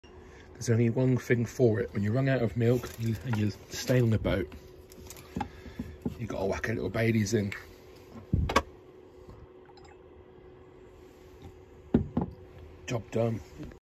Run out of milk…. What’s a man to do..???
A cheeky morning baileys always does the trick 😜

Run out of milk… so I HAD to..